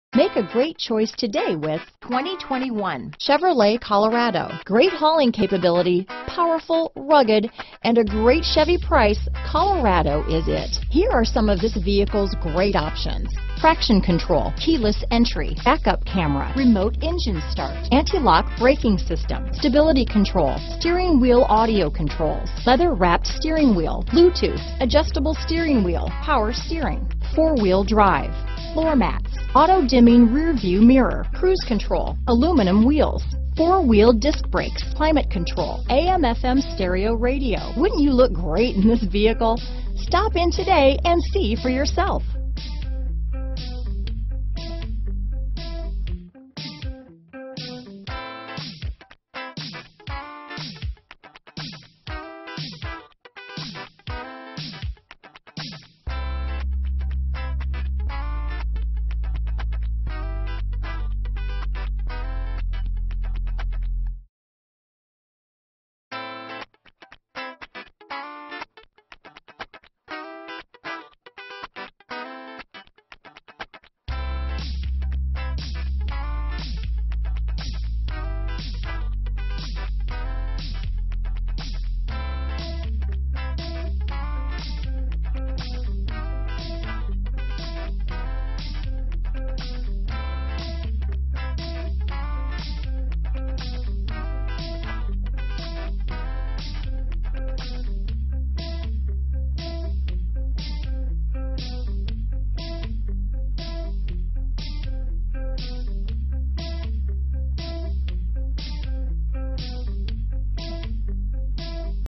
2021 Chevrolet Colorado Branson, Forsythe, Ozark, Kimberling City, Harrison, MO 215976
Summit White New 2021 Chevrolet Colorado available in Branson, Missouri at Pinegar Chevy Buick GMC Branson. Servicing the Forsythe, Ozark, Kimberling City, Harrison, MO area.
2021 Chevrolet Colorado 4WD Z71 - Stock#: 215976 - VIN#: 1GCGTDEN0M1215976